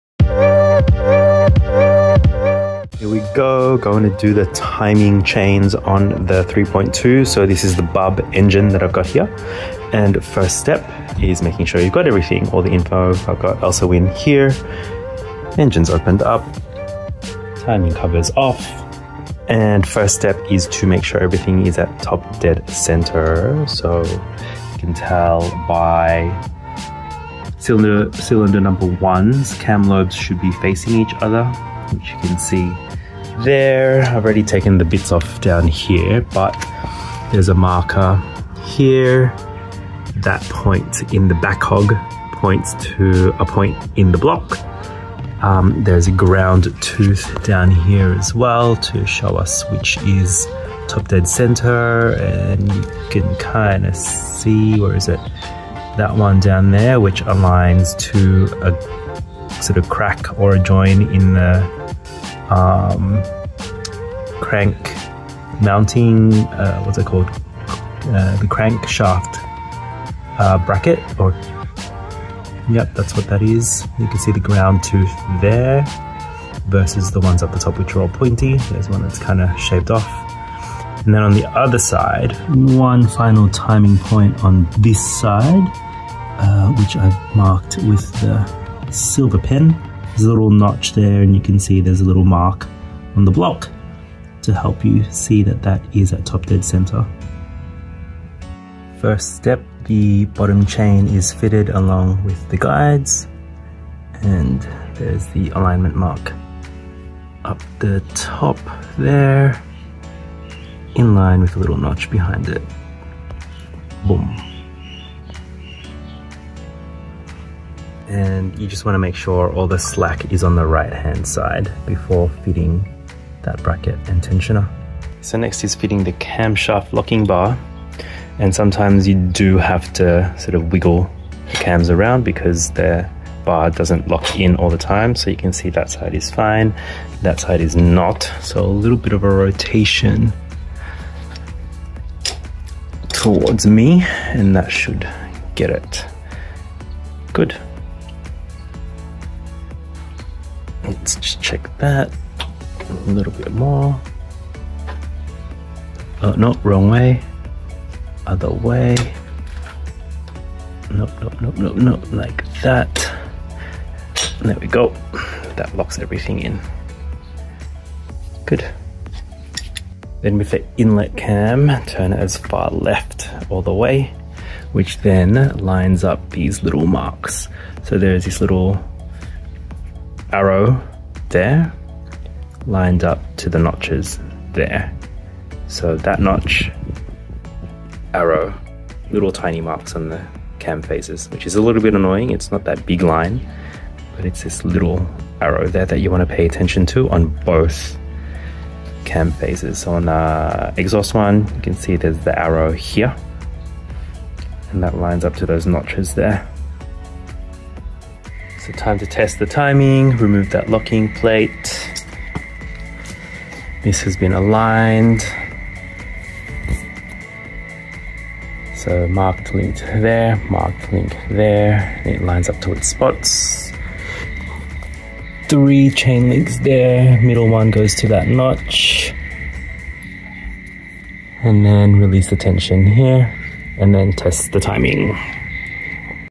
VW Bora VR6 3.2L R32 Swap! Tips for Perfect Timing Chain Setup #1 (Golf Mk4 platform)
This beautiful Jazz Blue VW Bora 2.8L VR6 had a  devastating hydrolock, so we decided it was time for a serious upgrade. We're swapping out the damaged 2.8L for a powerful 3.2L BUB engine from a Mk5 R32!

Here are some detailed videos for getting your timing chain PERFECT on the 3.2L BUB engine. No more guesswork! We'll show you the exact steps and key markings to ensure your timing is spot-on, preventing costly engine damage.

Whether you're a seasoned mechanic or a DIY enthusiast, this video is sure to help you gain confidence in DIYing your timing chains and guides.

- Ian